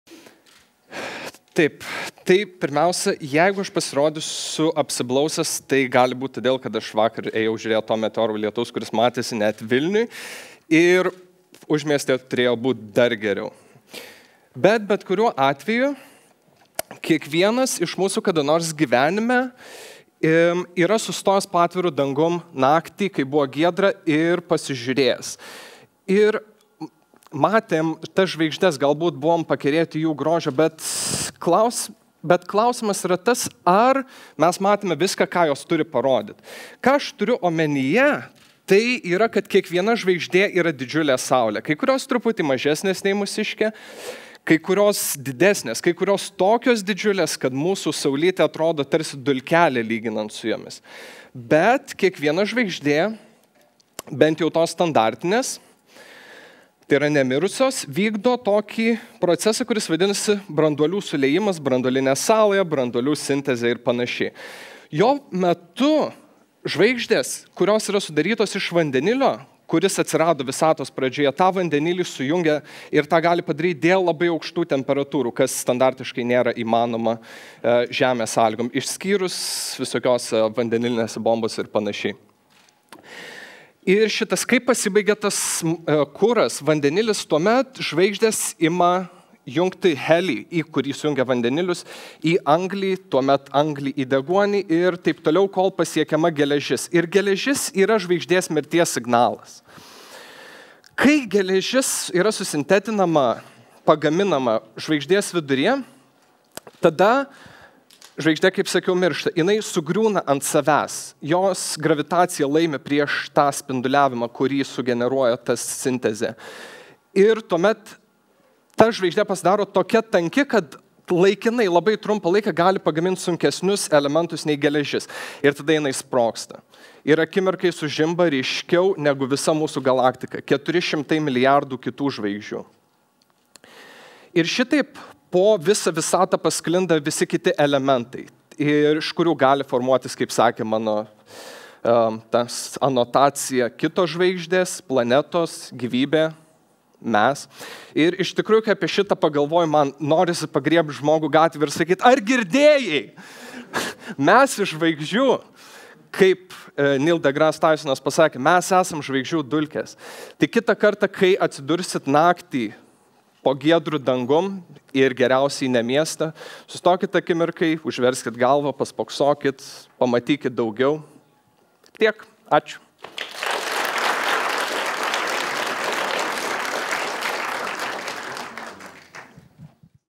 Gediminas Drabavičius, Šlovės laboratorija, 2014
Gedimino Drabavičiaus, Vilniaus Gedimino technikos universiteto bioinžinerijos bakalauro studento, kalba „ Žvaigždės, mirdamos didingomis supernovomis, po visatą paskleidė medžiagas reikalingas gimti kitoms žvaigždėms, planetoms, mums."